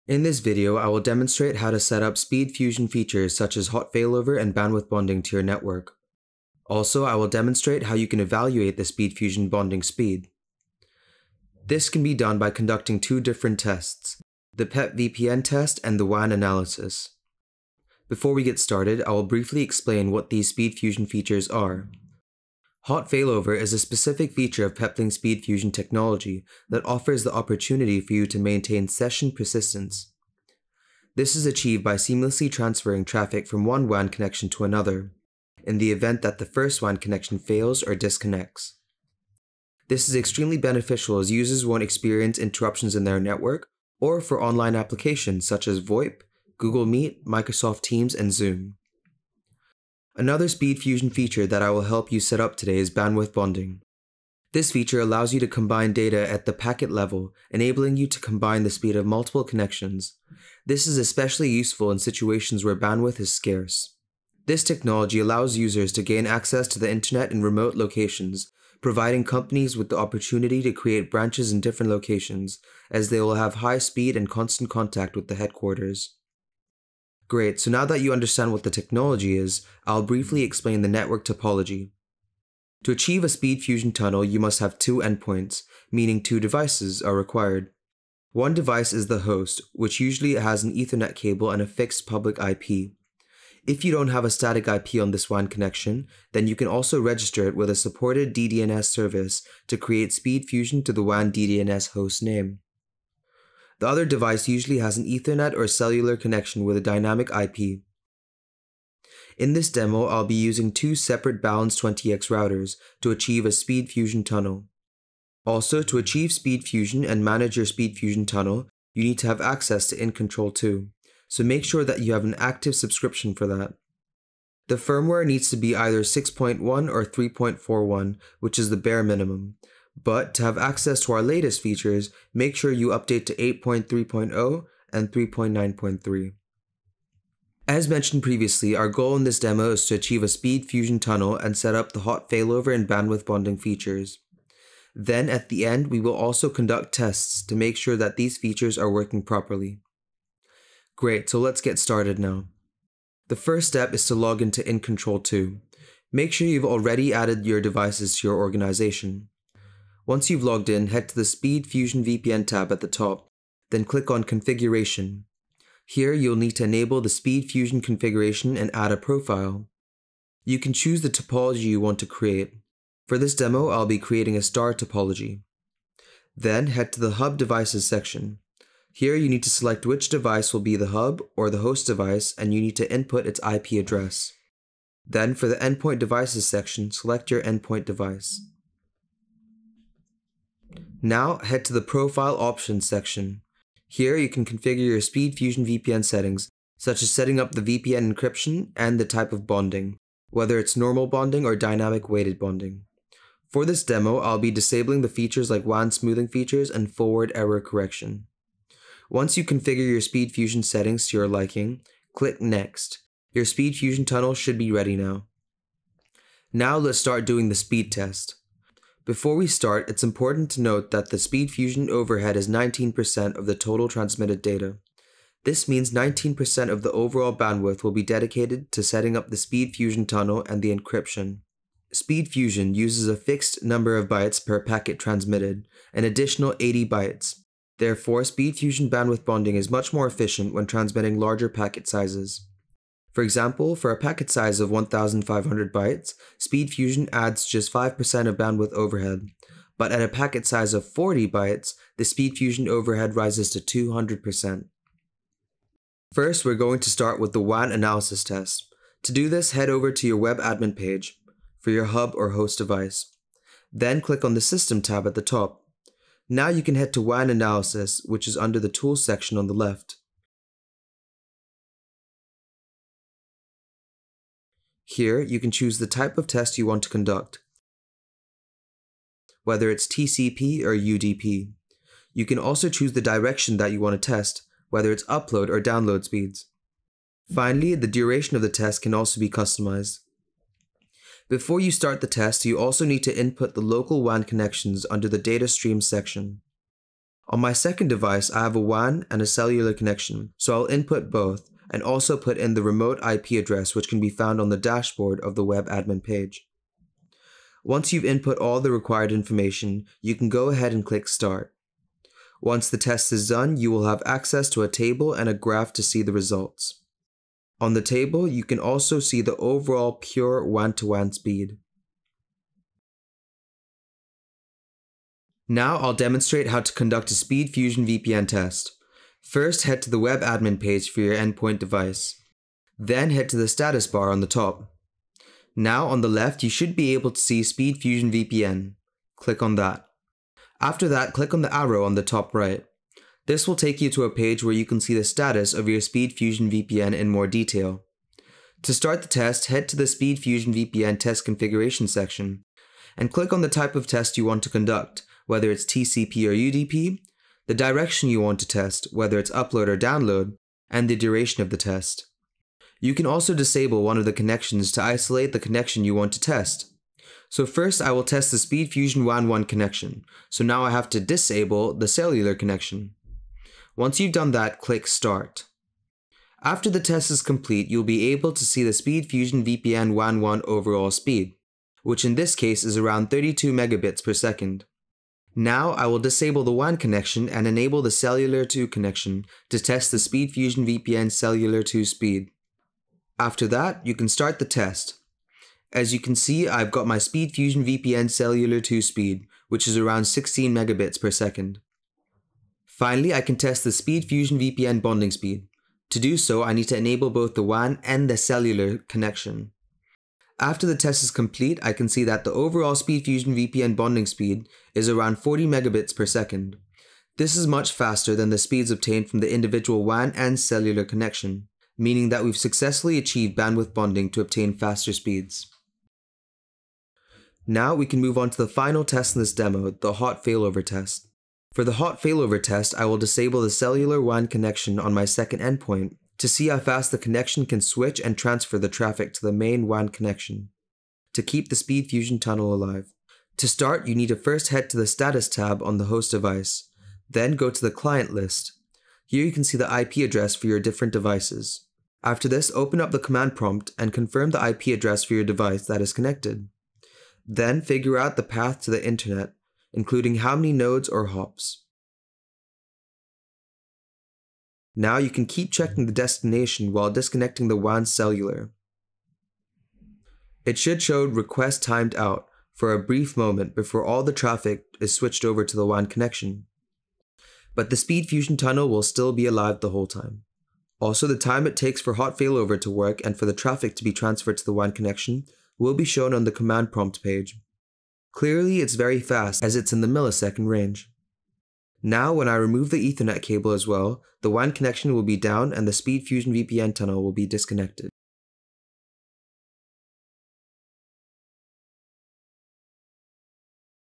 Support | How to Set up and Test SpeedFusion Features
Learn how to set up SpeedFusion (SF) features such as Hot Failover and Bandwidth Bonding to your network. Also, understand how to evaluate the SF bonding speed. This can be done by conducting the PepVPN test, and the WAN analysis.